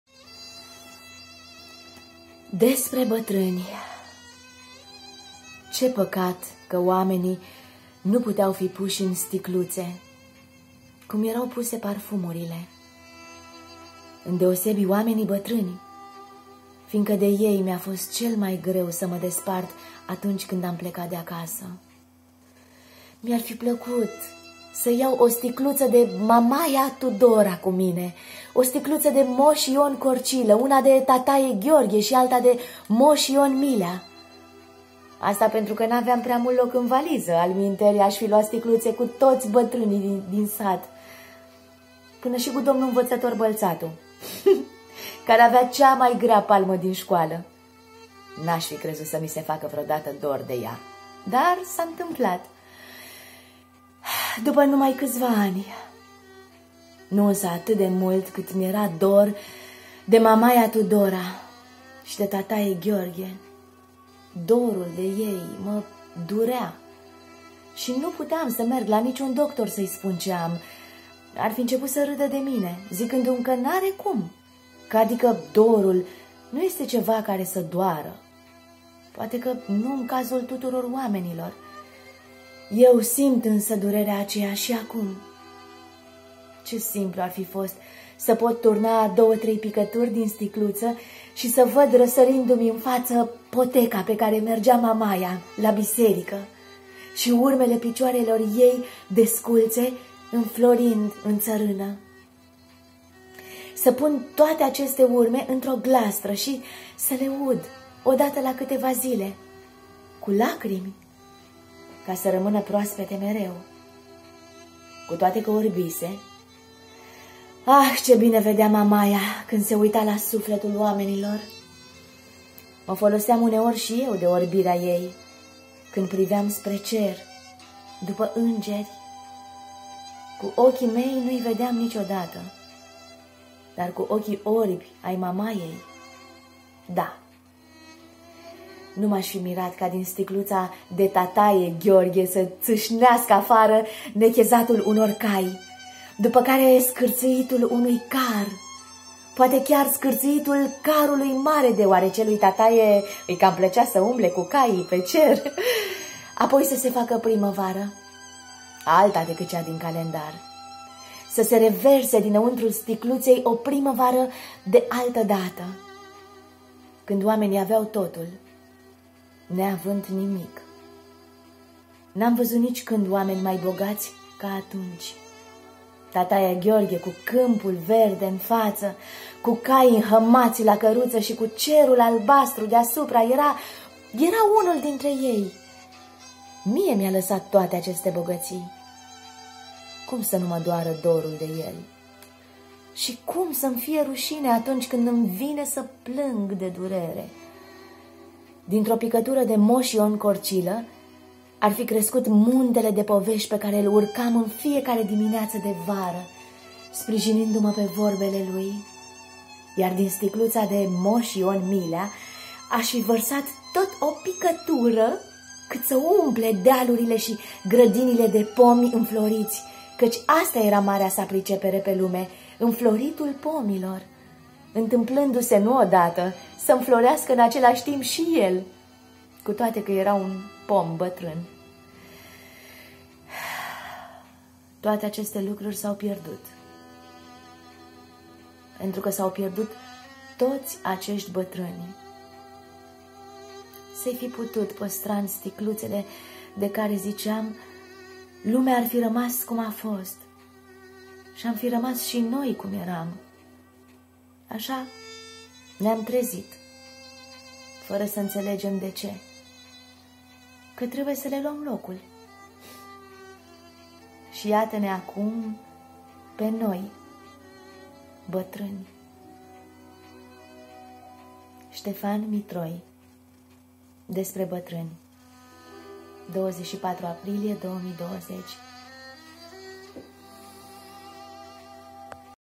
"Despre bătrâni" de Ștefan Mitroi
DespreBătrâni scris de Stefan Mitroi ... articolul , poemul de Dor de azi din ziarul Lumina...
Mai actual ca niciodată...acest dordebătrâni...in lumina ultimelor evenimente... vă rog dăruiți 4 minute din viața dvs și ascultați o declarație de iubire minunată pentru "bătrânii" din viețile noastre..❤️
povestiinizolare pentru o mai bună stare spirit ❤️